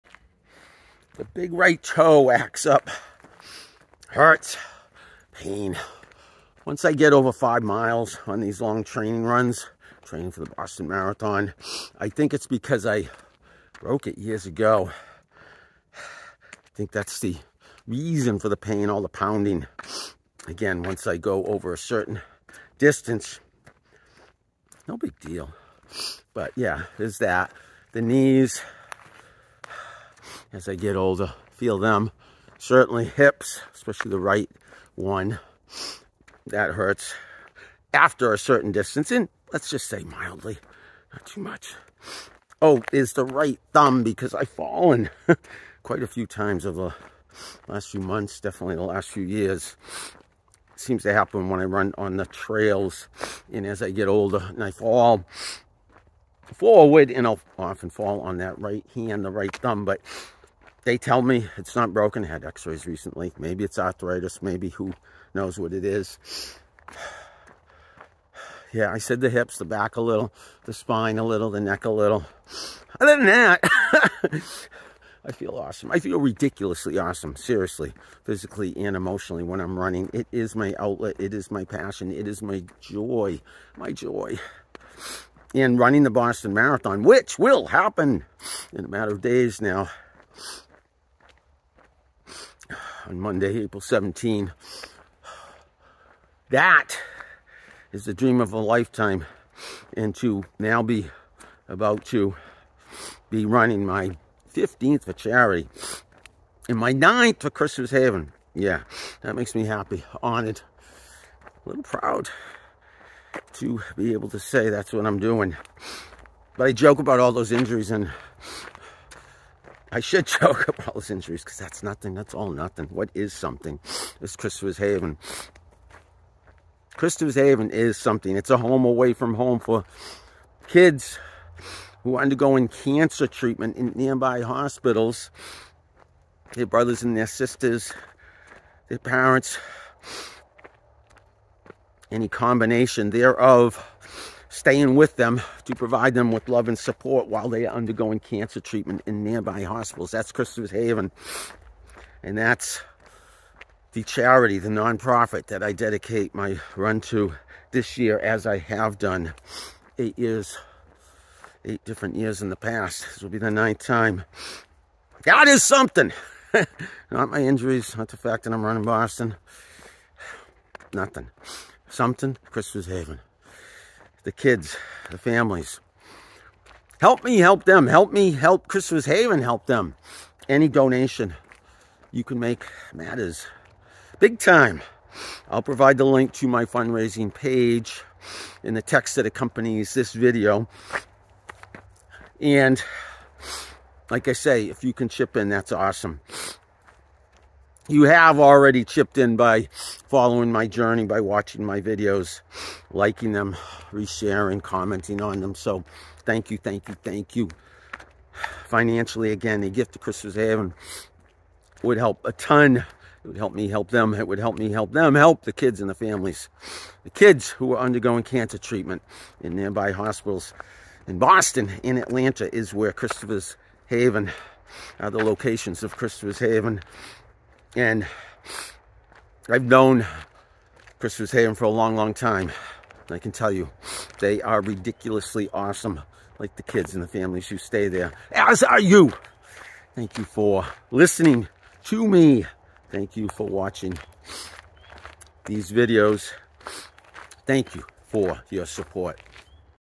Running the Boston Marathon - Again - for Christopher's Haven
Running the Boston Marathon. Again. Dedicating it to Christopher's Haven. Again. One week from today.